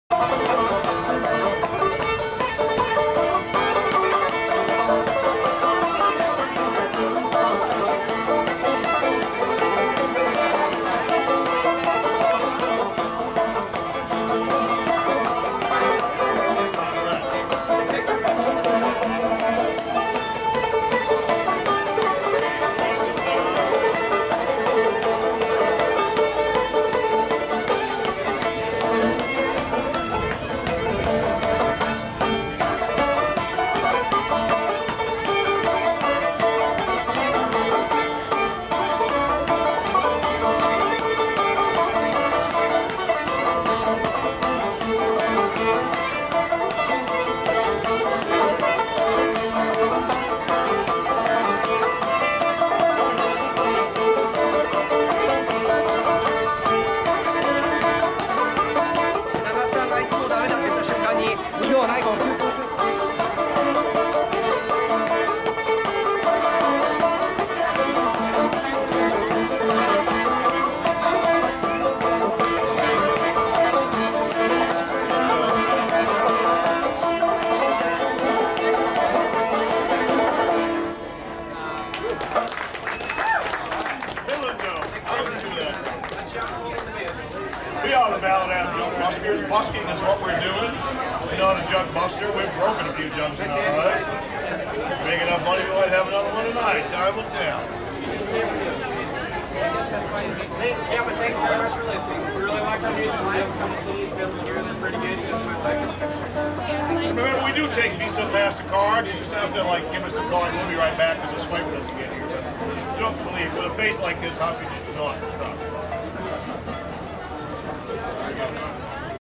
Starbucks street musicians in Pike Place Market
If you haven't seen these guys performing outside of Starbucks in Pike Place Market in Seattle it's worth the trip. Lots of great energy. Posted with permission - their sign says "If you like us enough to take our picture then like us enough to leave a dollar." I left a dollar.